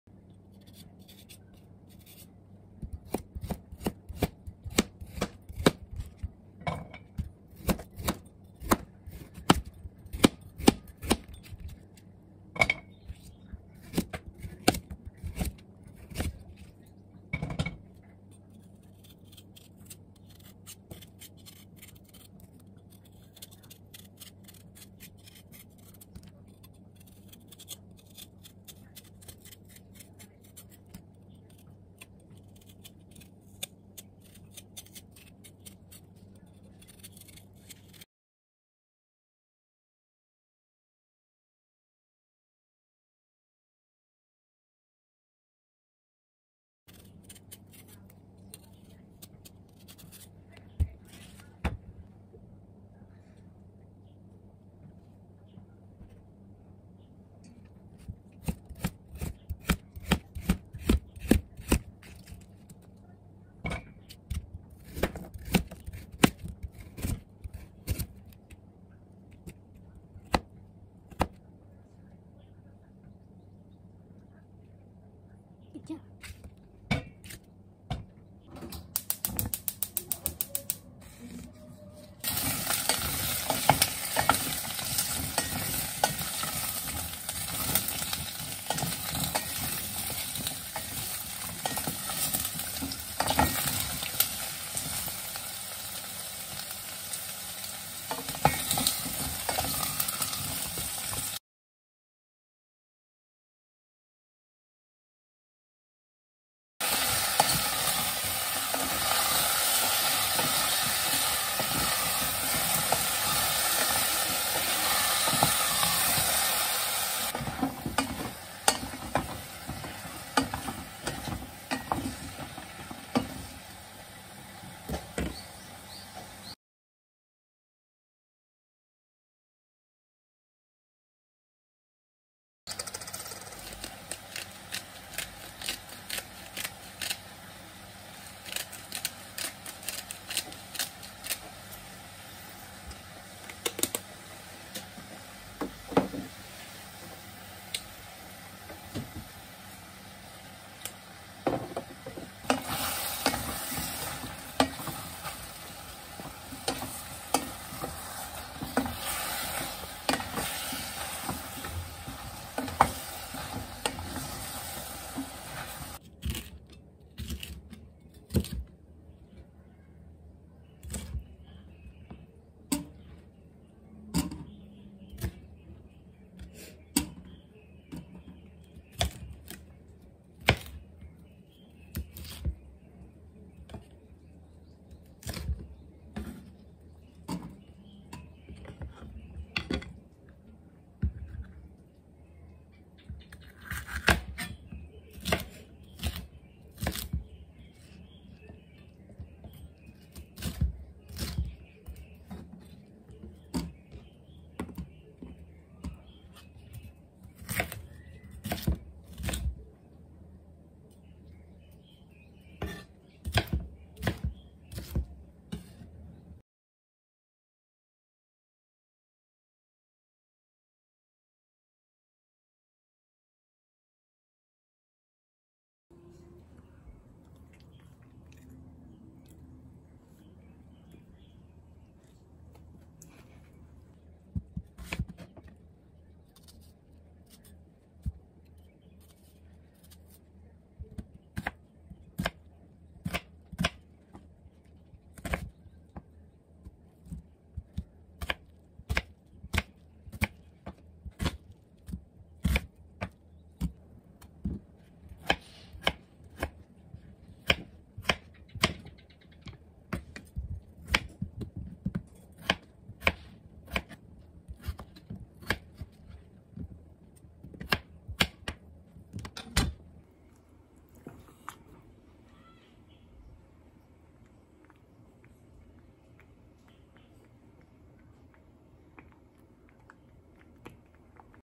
父の日エピソード カレーを作る vlog 手作り大好き 暮らし 日記
今日は今年あった父の日エピソードをお話ししました。
こんな事は初めてです。
思い出す度に笑いそうです😂

時。